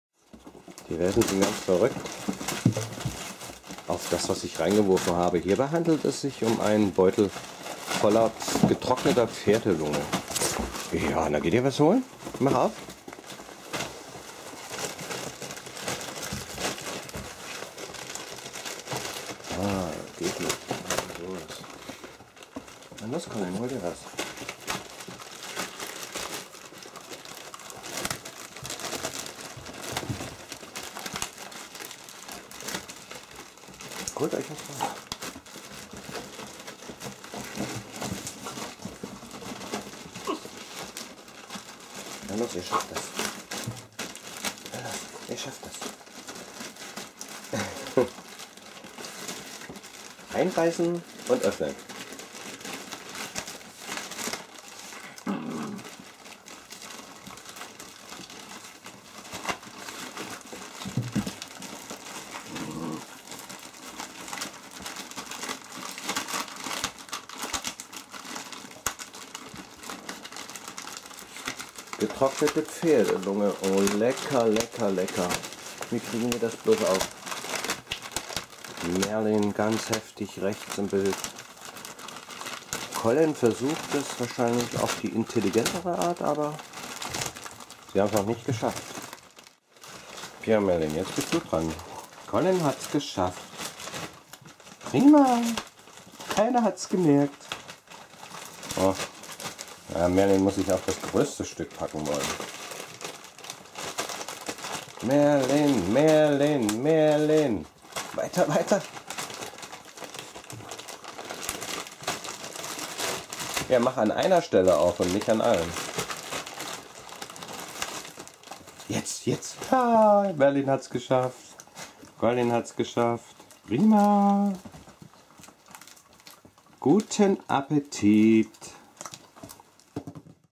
Dalmatiner vdS C-Wurf - Pferdelunge
Collin und Charming Merlin bekommen zur Selbstbedienung einen Beutel mit getrockneter Pferdelunge gereicht. Aber wie aufbekommen? Jetzt wird es spannend. Viel Spaß beim Zuschauen.